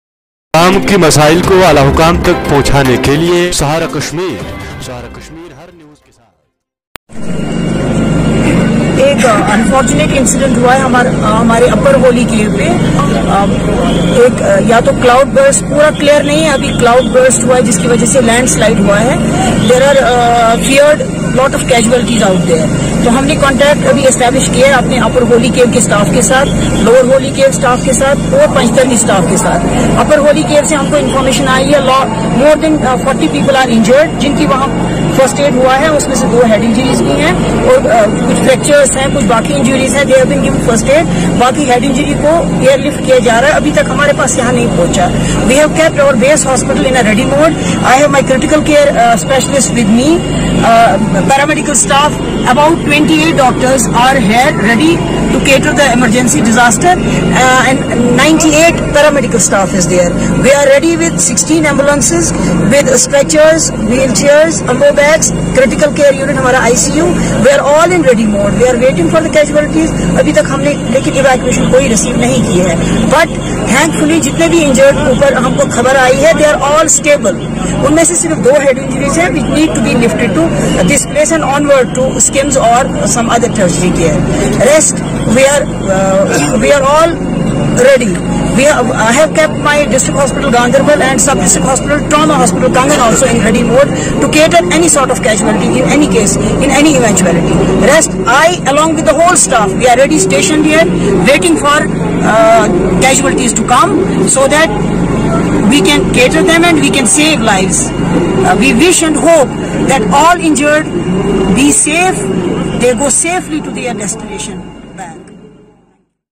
CMO Ganderbal apprising about the current overall health preparedness regrading the unfortunate
CMO Ganderbal apprising about the current overall health preparedness regrading the unfortunate natural disaster that happened at Upper Holy Cave. All arrangements upto the mark and in ready mode